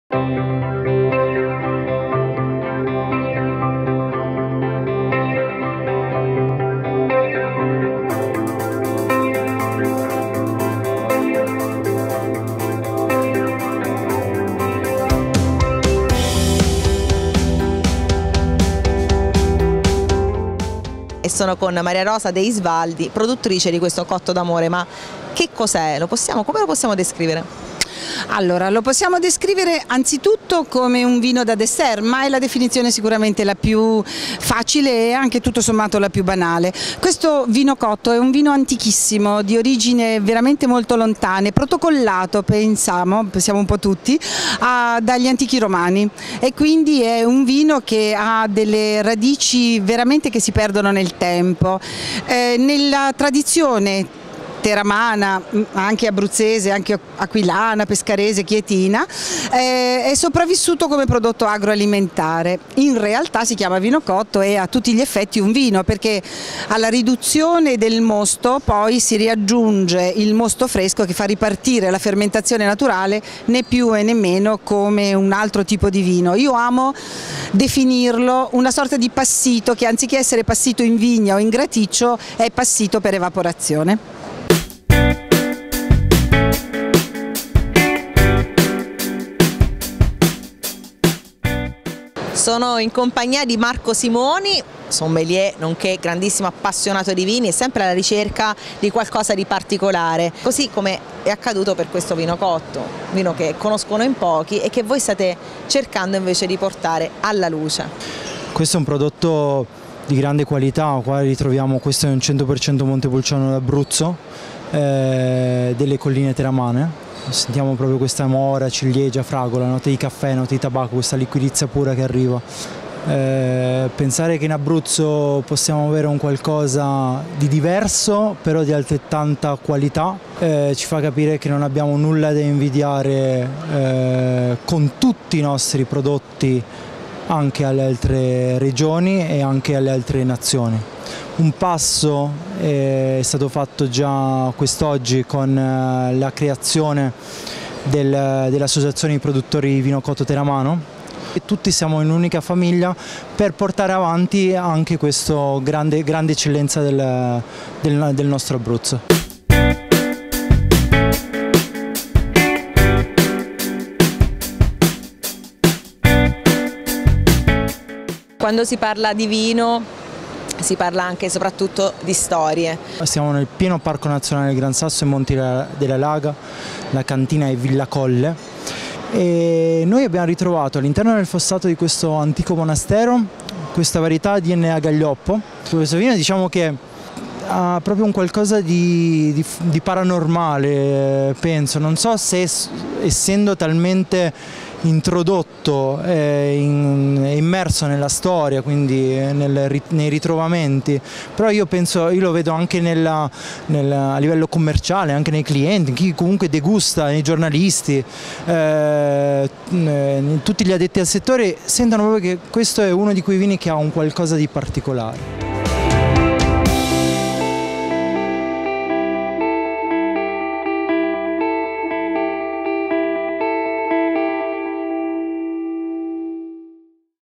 Vinitaly 2018 - L' Azienda Villa Colle racconta la storia del vino Cotto D'amore
Mariarosa dei Svaldi dell'azienda Villa colle ci racconta le caratteristiche del cotto d'amore, un vino ricco di storia e tradizione prodotto nel Teramano. A descriverci invece le peculiarità e alcuni aneddoti affascinanti il sommelier Marco Simoni.